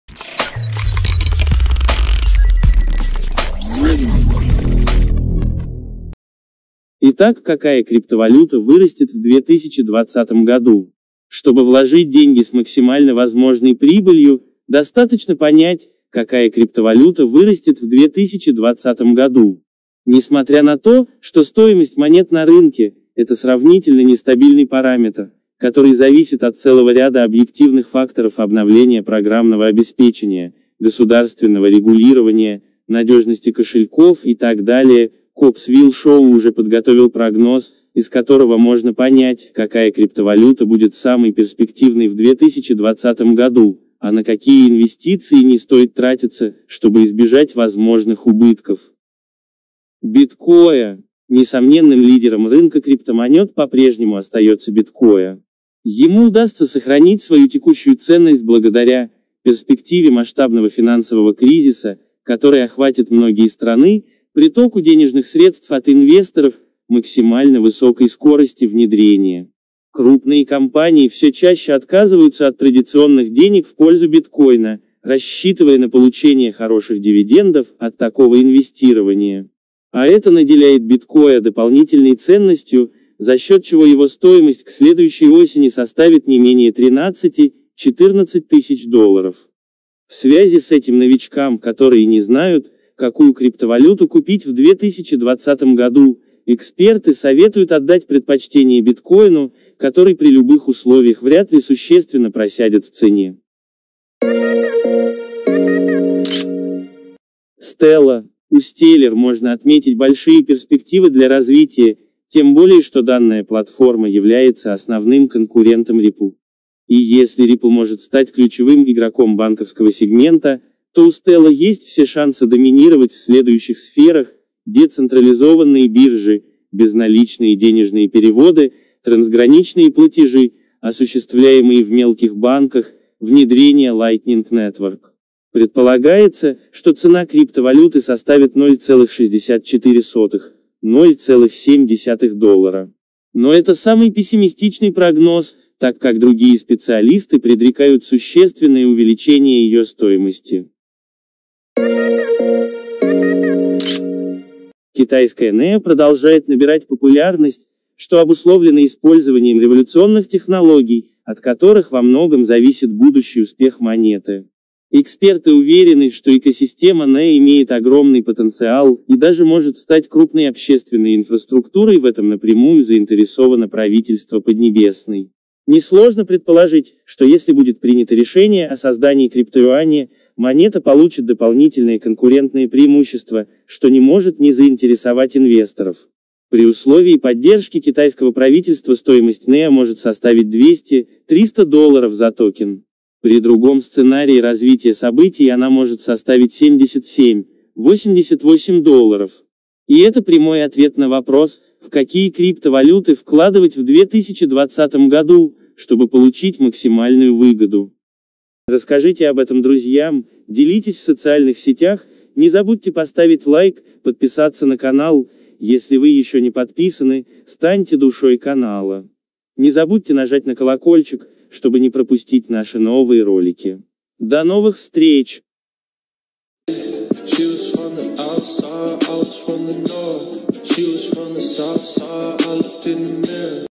НА ЭТИХ КРИПТОВАЛЮТАХ ВЫ ЗАРАБОТАЕТЕ В 2020 ГОДУ! ВНИМАНИЕ!
Итак, Какая криптовалюта вырастет в 2020 году! Чтобы вложить деньги с максимально возможной прибылью, достаточно понять, какая криптовалюта
 вырастет в 2020 году.
 Несомненным лидером рынка криптомонет по-прежнему остается биткоин. Ему удастся сохранить свою текущую ценность благодаря: перспективе масштабного
 финансового кризиса, который охватит многие страны; притоку денежных средств от инвесторов; максимально высокой скорости внедрения. Крупные
 компании все чаще отказываются от традиционных денег в пользу биткоина, рассчитывая на получение хороших дивидендов от такого инвестирования.
А это наделяет биткоин дополнительной ценностью, за счет чего его стоимость к следующей осени составит не менее 13, 14 тысяч долларов.  В связи с этим
новичкам, которые не знают, какую криптовалюту купить в 2020 году, эксперты советуют отдать предпочтение биткоину, который при любых условиях вряд ли
существенно просядет в цене.

У Стеллар можно отметить большие перспективы для развития, тем более что данная платформа является основным конкурентом Рипл. И если Рипл может
стать ключевым игроком банковского сегмента, то у Stellar  есть все шансы доминировать в следующих сферах: децентрализованные биржи; безналичные
денежные переводы; трансграничные платежи, осуществляемые в мелких банках.
Китайская NEO продолжает набирать популярность, что обусловлено использованием революционных технологий, от которых во многом зависит будущий
успех монеты. Эксперты уверены, что экосистема NEO имеет огромный потенциал и даже может стать крупной общественной инфраструктурой в этом напрямую
 заинтересовано правительство Поднебесной. Несложно предположить, что если будет принято решение о создании криптоюаня, монета получит
дополнительное конкурентное преимущество, что не может не заинтересовать инвесторов. При условии поддержки китайского правительства стоимость
NEO может составить 200, 300 долларов за токен. При другом сценарии развития событий она может составить 77, 88 долларов. И это прямой ответ на вопрос,
в какие криптовалюты вкладывать в 2020 году, чтобы получить максимальную выгоду.